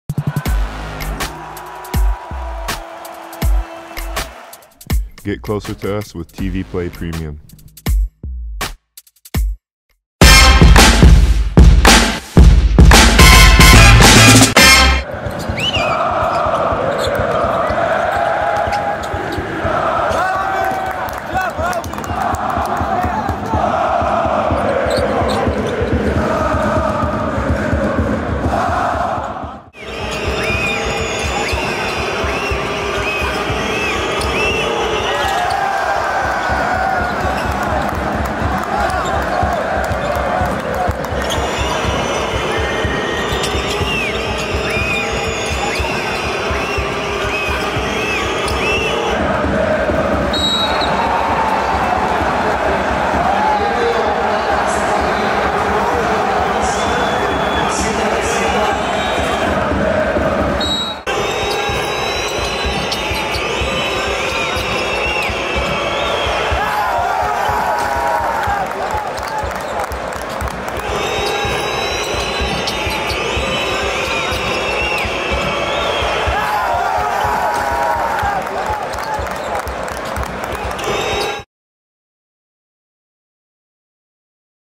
Zalgiris explodes with dunks in EuroLeague game vs Panathinaikos
Zalgiris Kaunas players explode with dunks in the EuroLeague game vs Panathinaikos Athens.